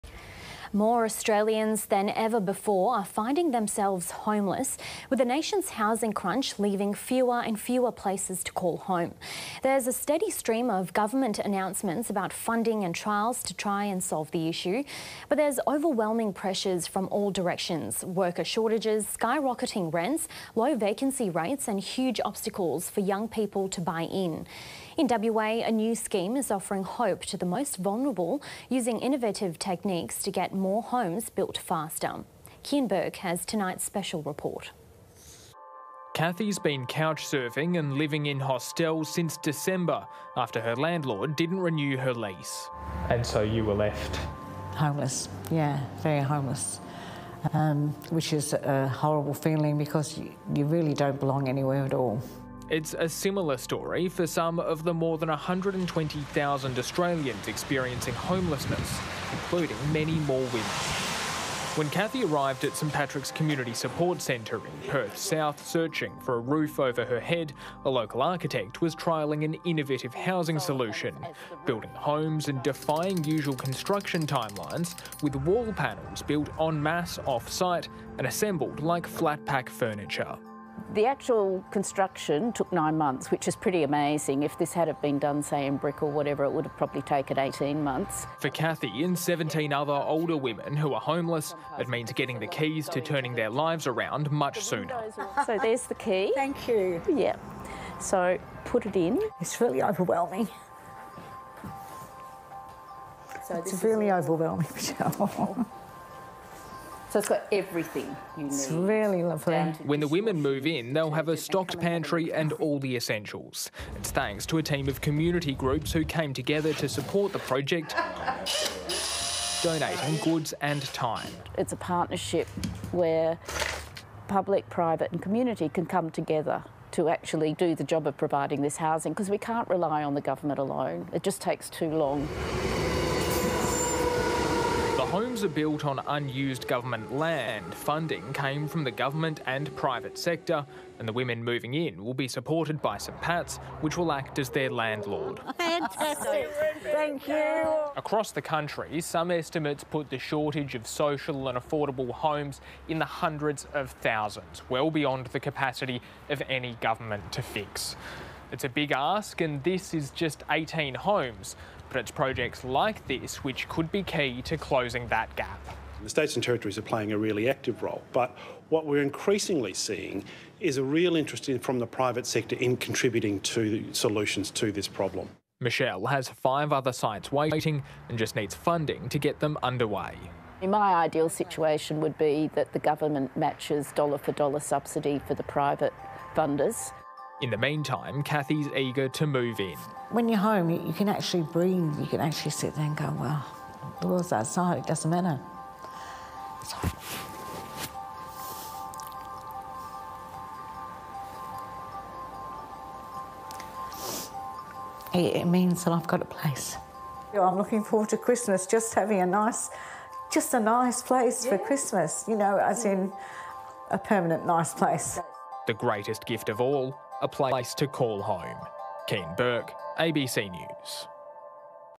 Retrospective: Tiny Homes For Little Women But Not Men
Retrospective: Tiny Homes For Women With Little But Not Homeless Men.

(Source: ABC Australia, WA - 10 Jun 2023)